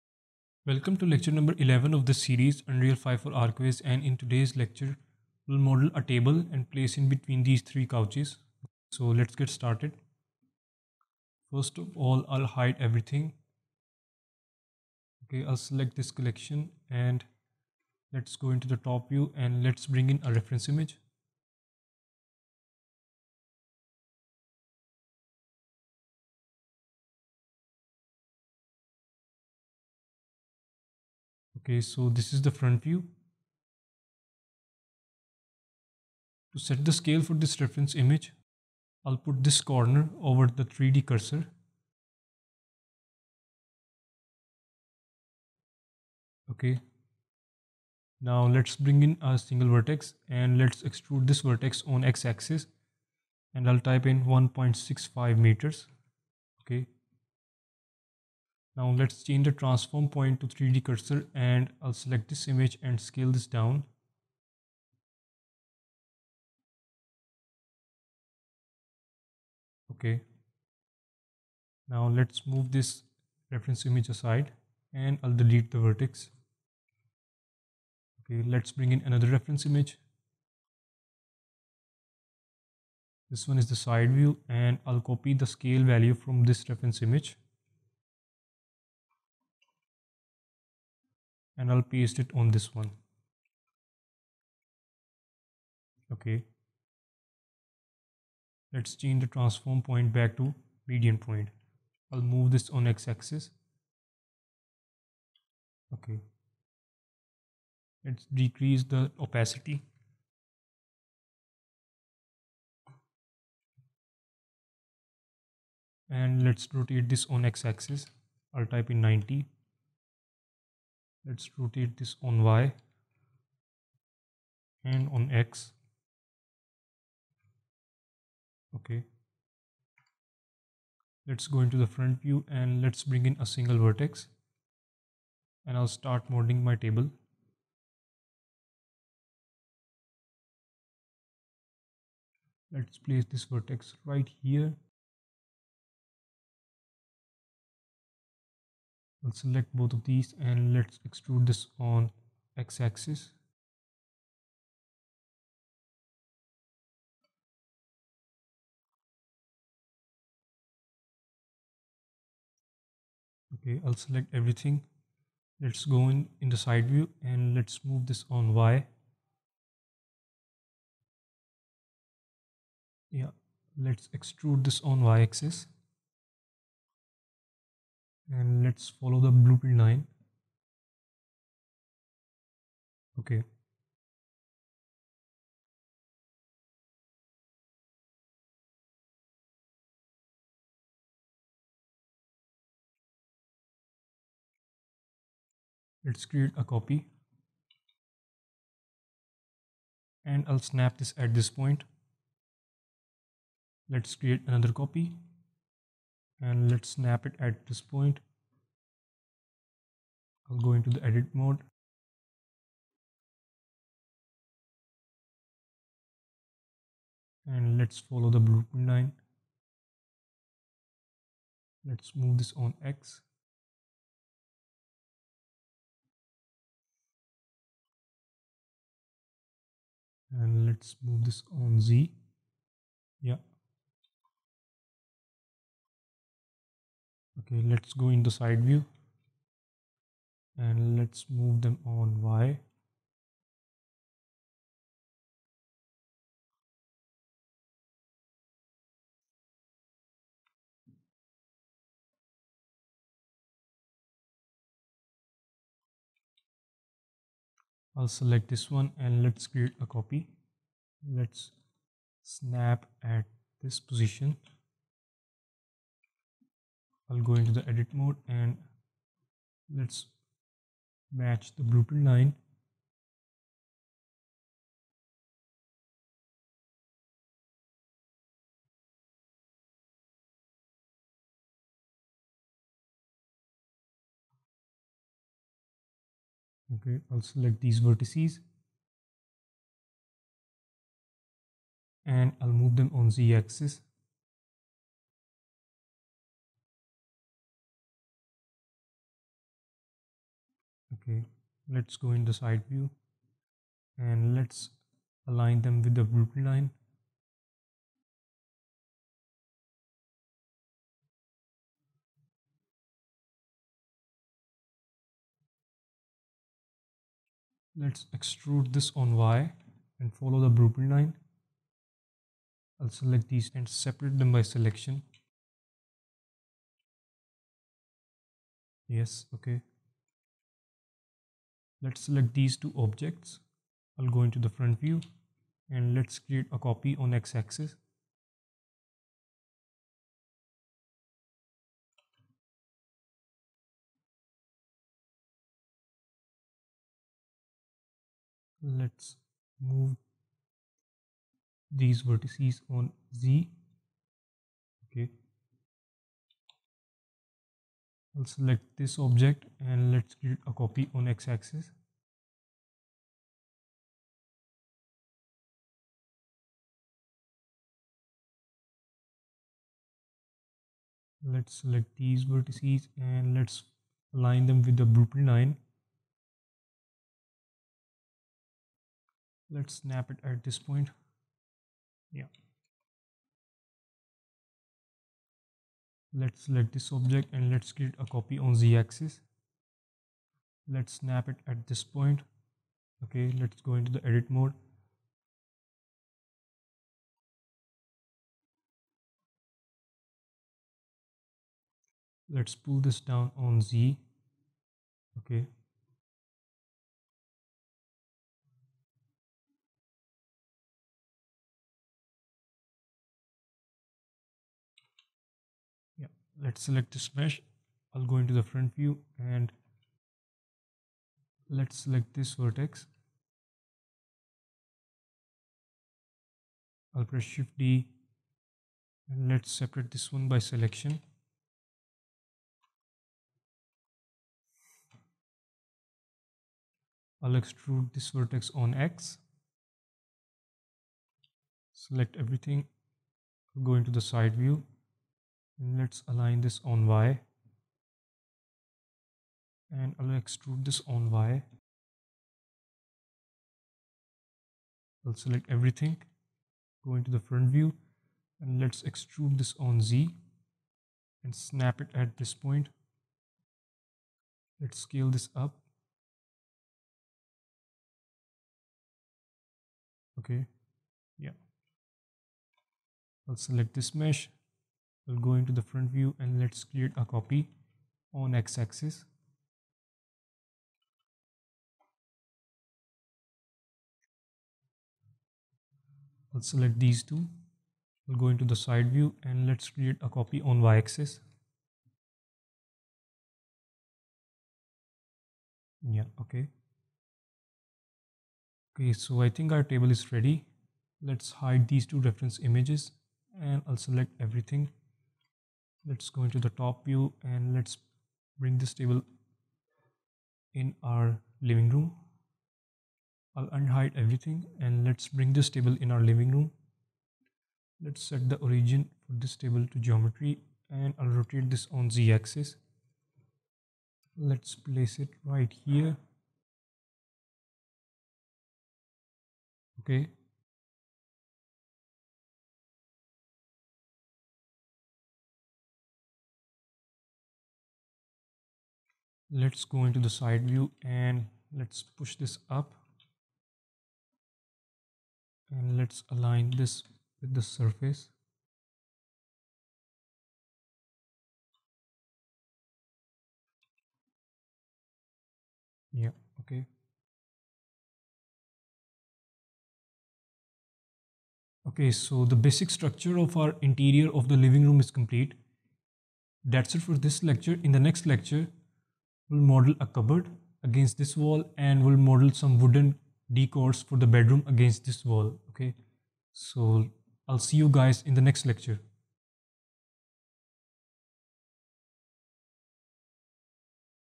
Unreal Engine 5.1 For Architectural Visualization - Modeling Table
Unreal Engine 5.1 For Architectural Visualization Modeling Table. 11th Class of Unreal Engine 5.1 for Architectural Visualization in which you'll learn How to Model a Table in Blender 3D Software for this Unreal Engine Archviz Course. If you're using any other software the process is pretty much the same.

Unreal Engine Version - Unreal Engine 5.1
Blender Version - 3.0.1